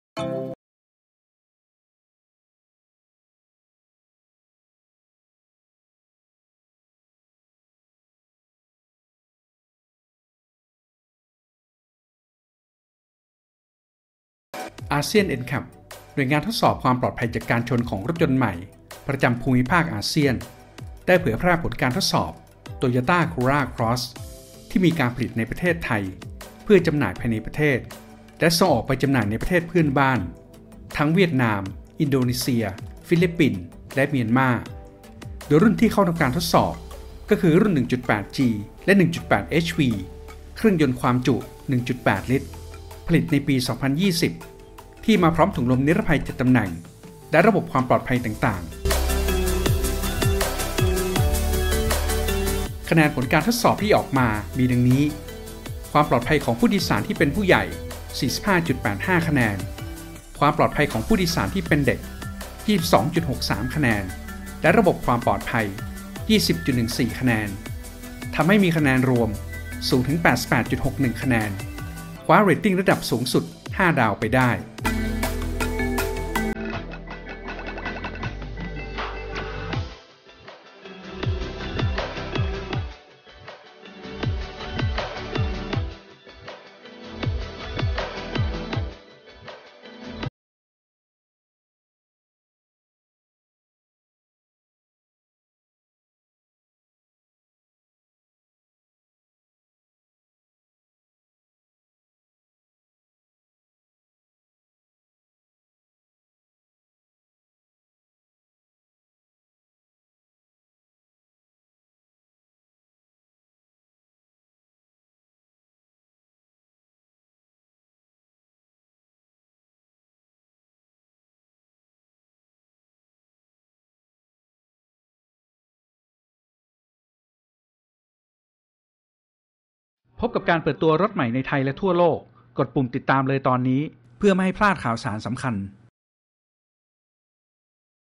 เผยผลการทดสอบ 2020 Toyota Corolla Cross 1.8G / HV ด้านความปลอดภัยจากการชน โดย ASEAN NCAP
เผยผลการทดสอบ All-New Toyota Corolla Cross 1.8G / 1.8HV 2020 โตโยต้า โคโรลล่า ครอส ด้านความปลอดภัยจากการชน โดย ASEAN NCAP

ASEAN NCAP หน่วยงานทดสอบความปลอดภัยจากการชนของรถยนต์ใหม่ ประจำภูมิภาคอาเซียน ได้เผยแพร่ผลการทดสอบ Toyota Corolla Cross ที่มีการผลิตในประเทศไทย เพื่อจำหน่ายภายในประเทศ และส่งออกไปจำหน่ายในประเทศเพื่อนบ้าน ทั้งเวียดนาม อินโดนีเซีย ฟิลิปปินส์ และเมียนม่าร์ โดยรุ่นที่เข้าทำการทดสอบ ก็คือ รุ่น 1.8G และ 1.8HV เครื่องยนต์ ความจุ 1.8 ลิตร ผลิตในปี 2020 ที่มาพร้อมถุงลมนิรภัย 7 ตำแหน่ง และระบบความปลอดภัยต่างๆ ติดตามช่องของเราทาง Facebook ได้ที่